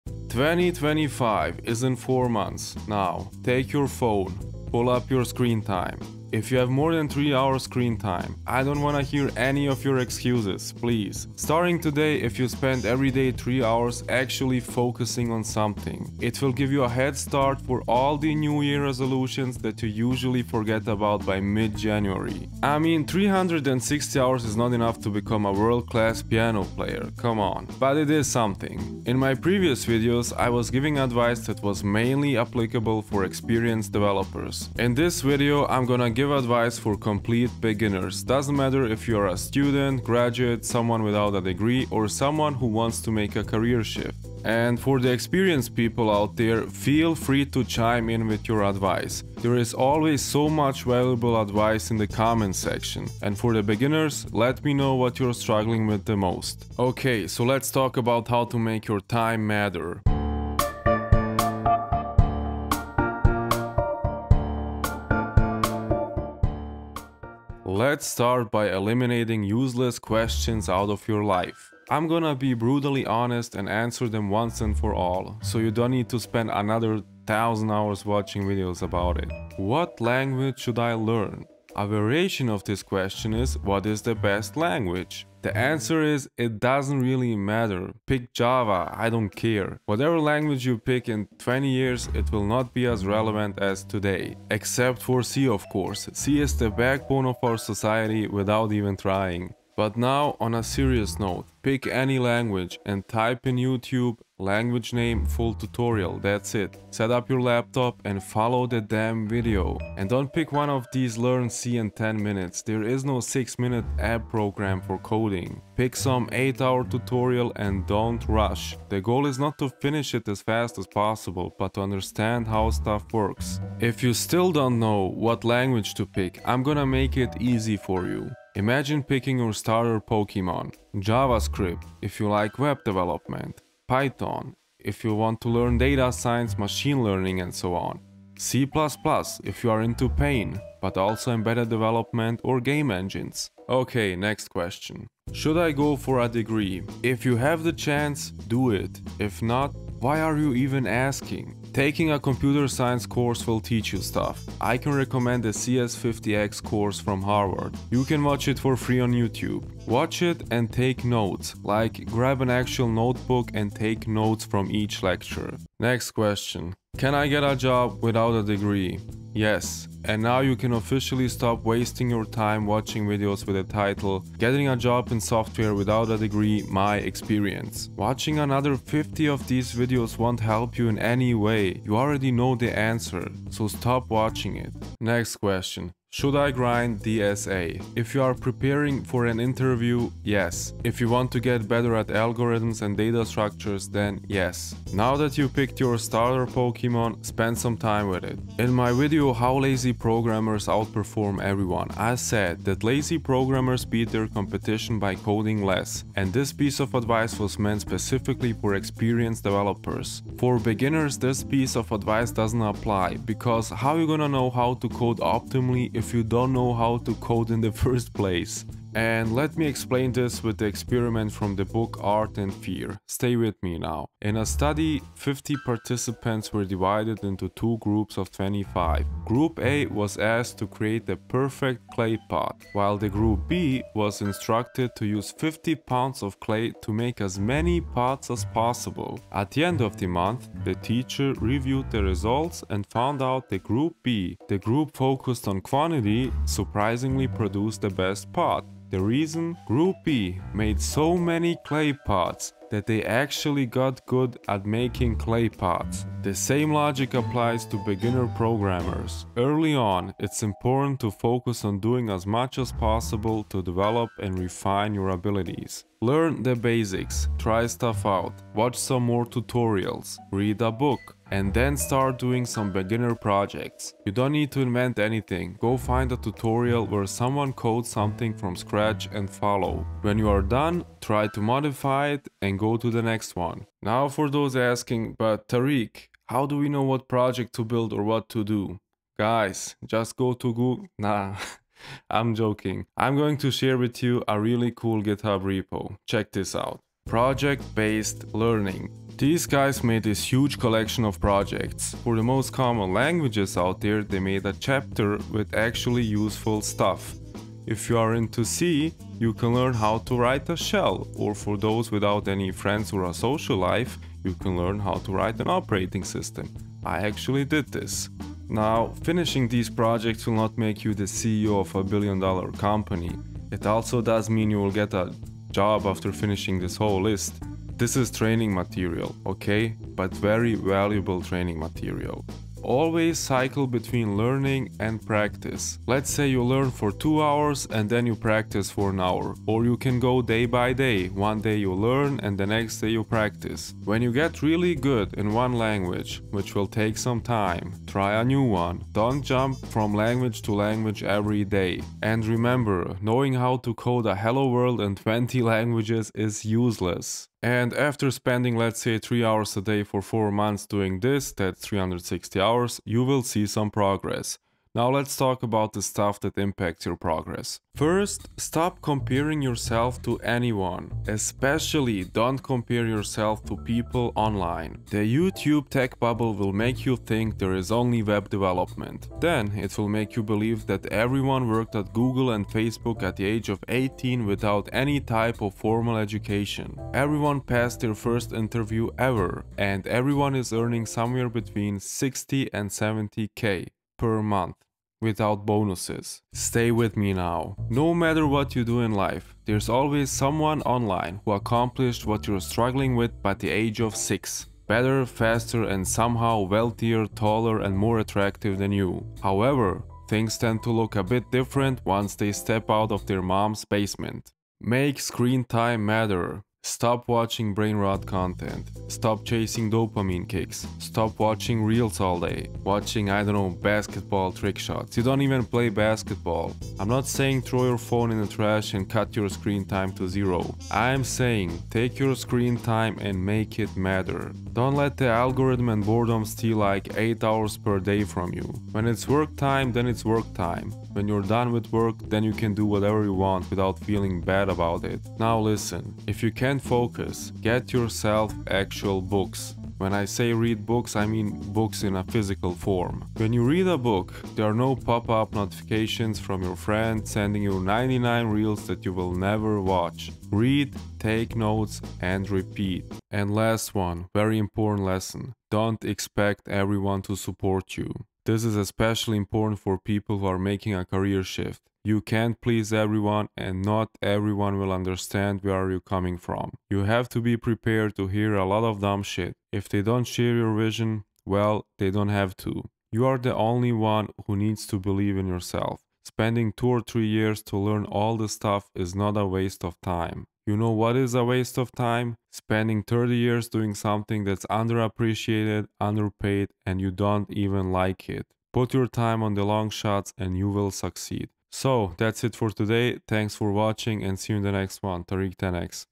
Want to Start Coding? The Only Video You Need (+with resources)
In this video I will give brutal but honest advice for people who want to get into software development. I will answer the most common but useless questions. Then I will provide you actually valuable learning material.

I am planning on doing more in-depth code reviews of interesting code bases. Let me know what you would like me to review and make a video about it.